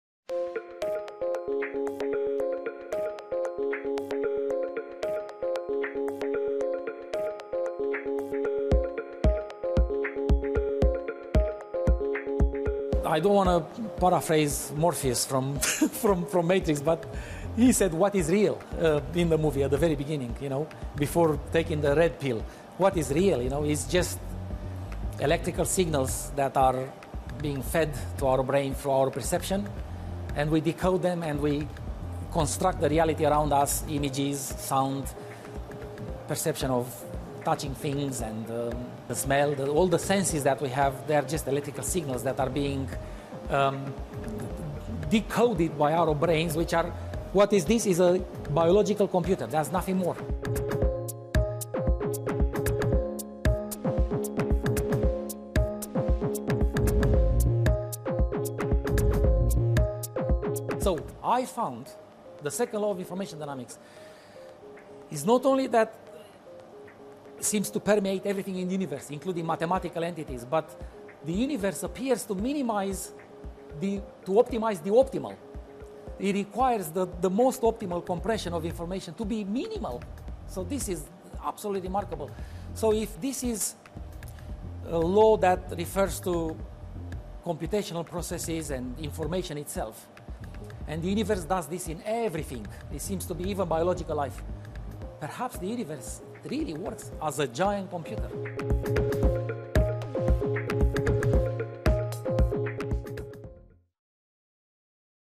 Are we living in a computer simulation? This physicist says his study supports the theory
Dr Melvin Vopson published the research in AIP Advances which suggests that the universe behaves just like a computer, ordering and deleting unnecessary information.